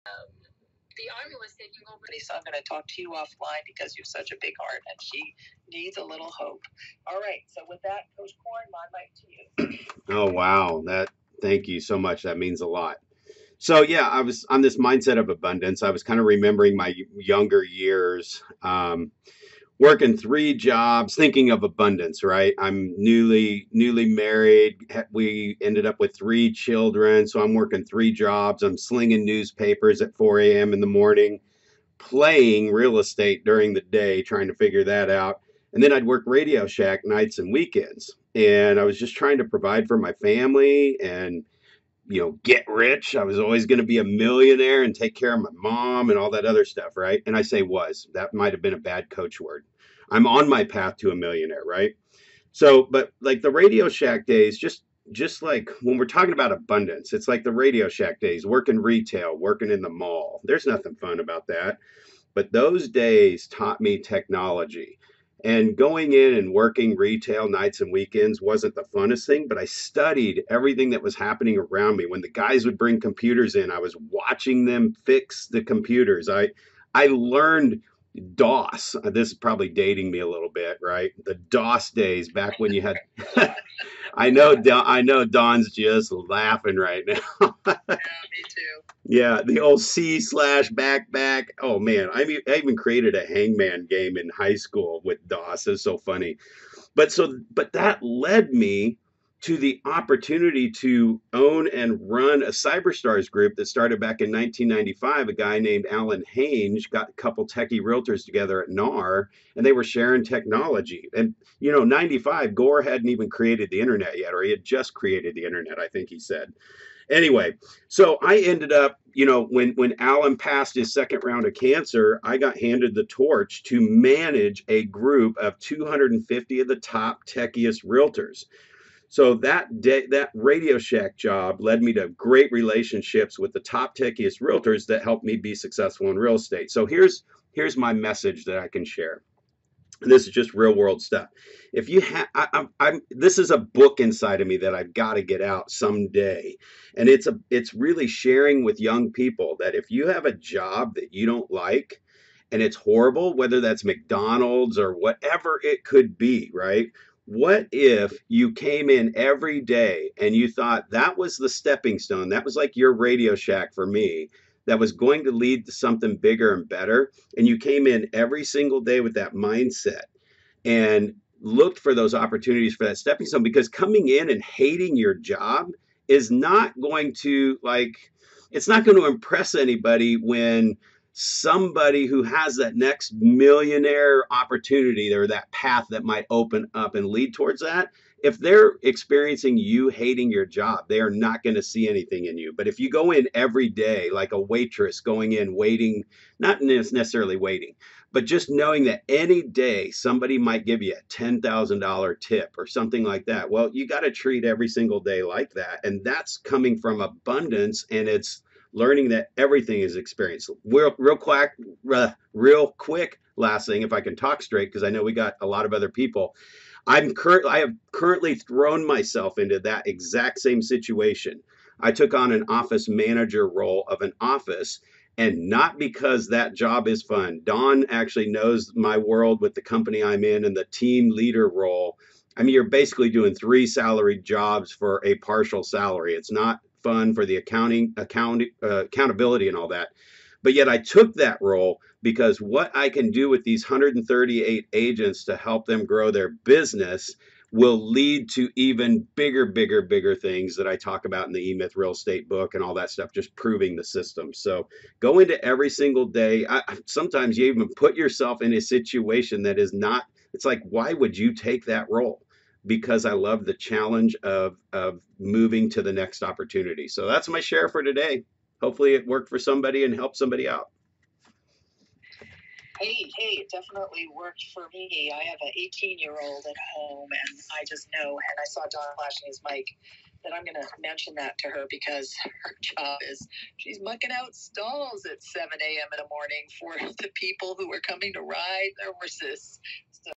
Living and Abundant Life EVERY Day no matter what job you have
Today is a share on how-to live-in abundance every day no matter where you are, where you work or whatever is happening. If you treat every day as a steppingstone to your perfect life, what can you take from every day to learn and move you towards that perfect life. Dragging through every day will not create the magnetic force to attract the next opportunity, relationship, etc that will keep you on your path towards your perfect life.